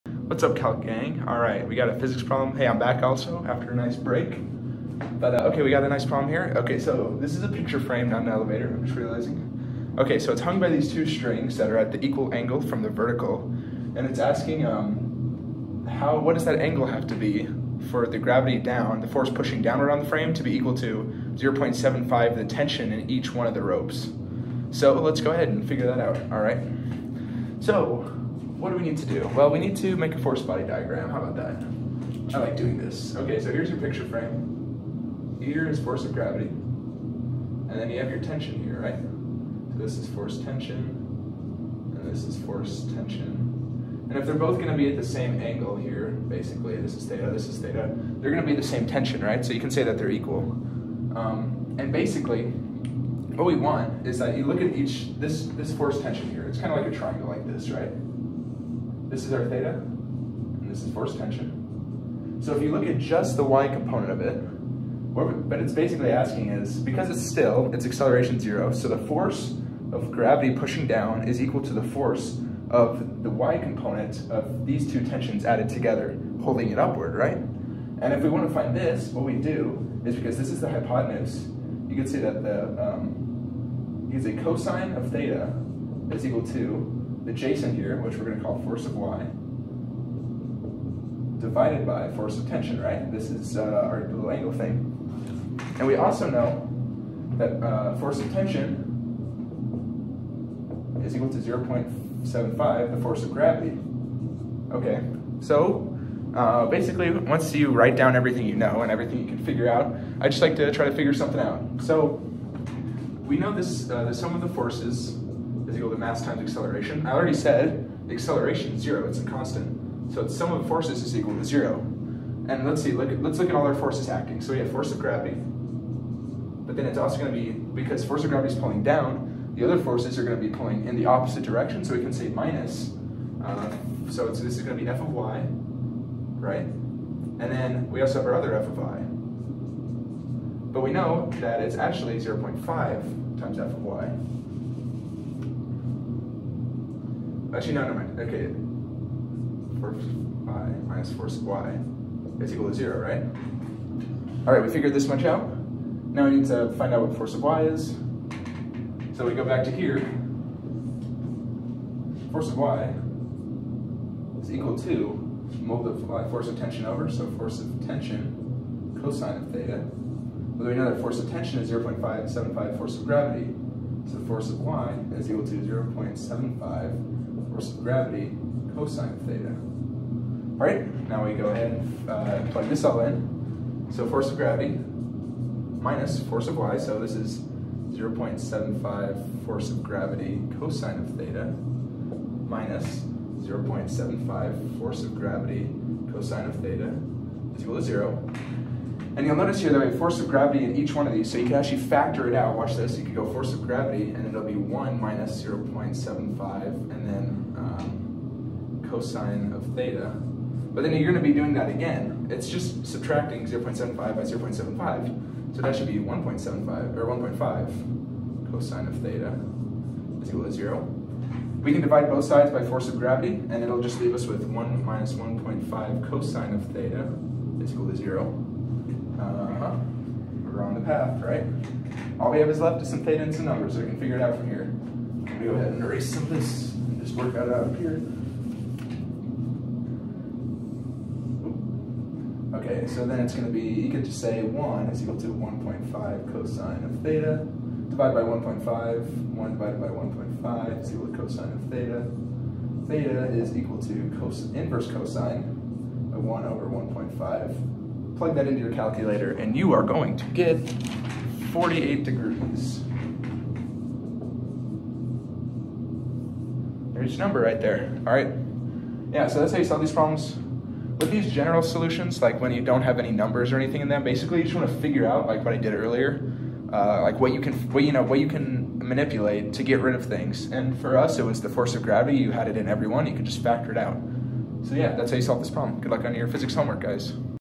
A picture frame hung against a wall is suspended by two wires - Problem 5.5
A picture frame hung against a wall is suspended by two wires attached to its upper corners. If the two wires make the same angle with the vertical, what must this angle be if the tension in each wire is equal to 0.75 of the weight of the frame? (Ignore any friction between the wall and the picture frame.)